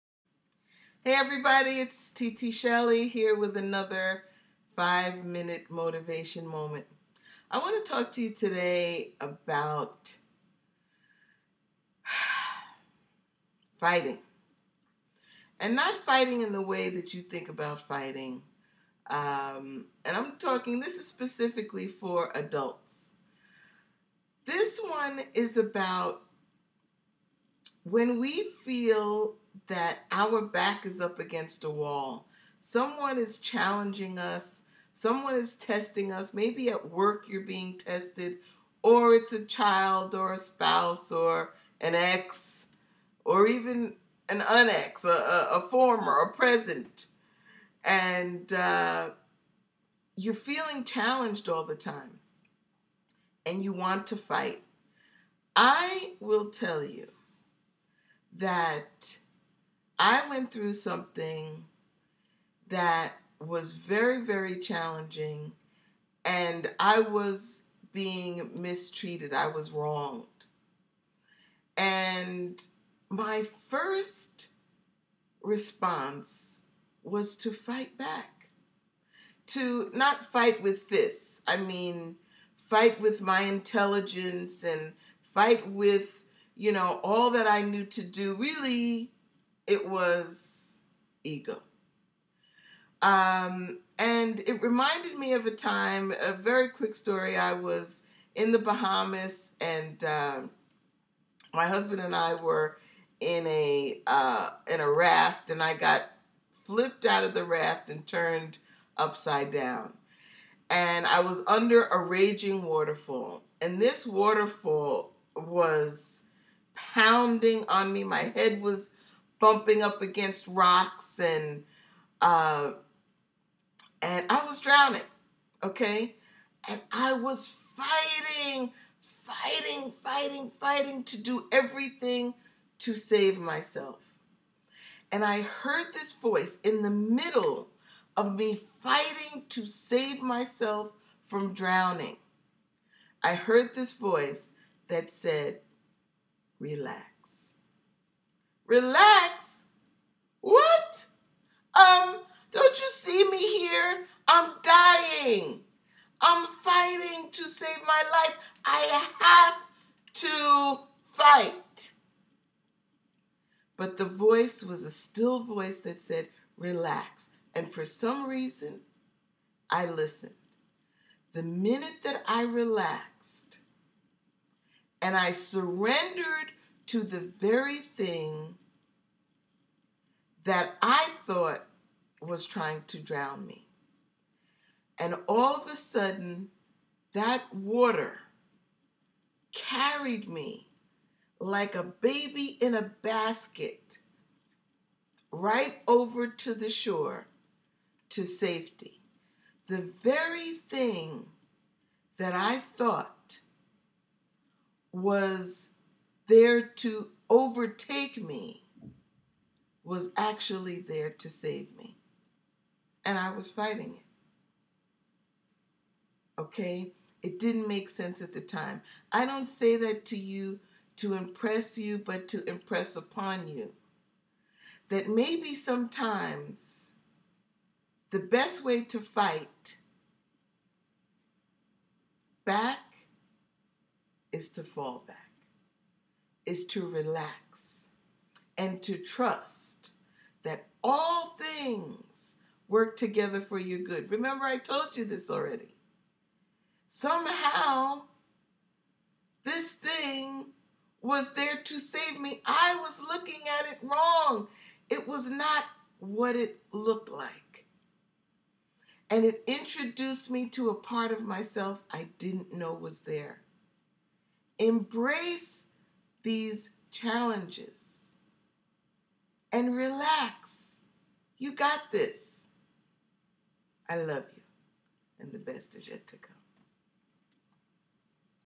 Fight back or Fall Back, the most effective way to Fight
Famed Life Strategist Shelley Roxanne with another 5-minute motivation moment